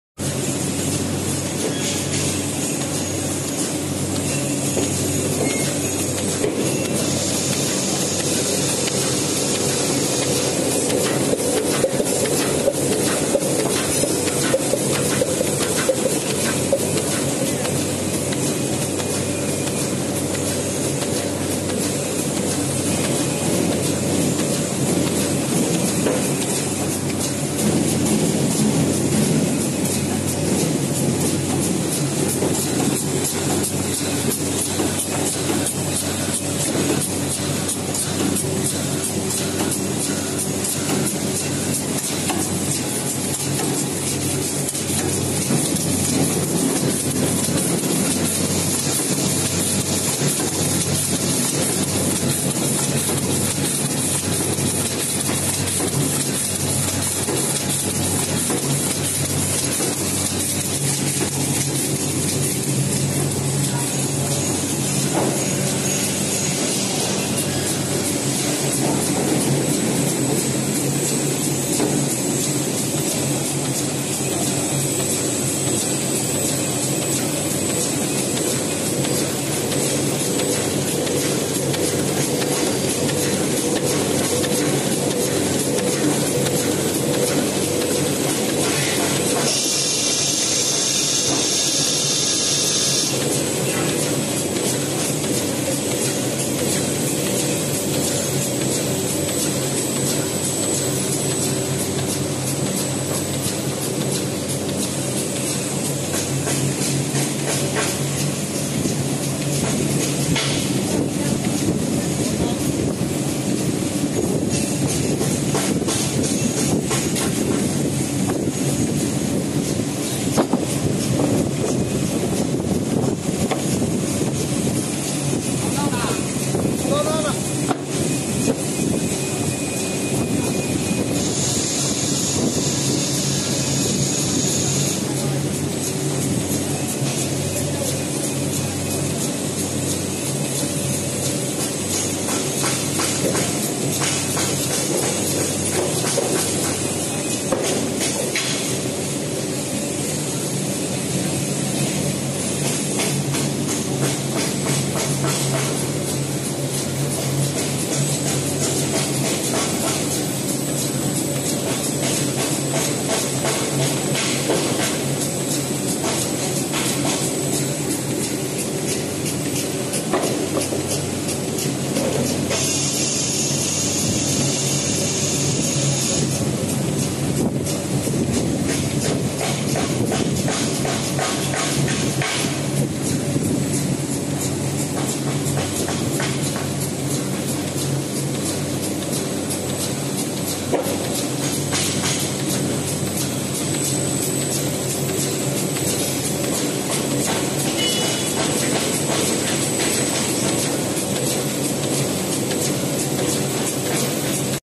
Automatic high speed cardboard to cardboard cardboard to corrugated board flute laminating machine
This video shows cardboard to cardboard lamination, both top and bottom sheets are cardboard. Machine can also do cardboard to corrugated board.

Andy Yang
Wechat: zouyuan895
Skype: innovo4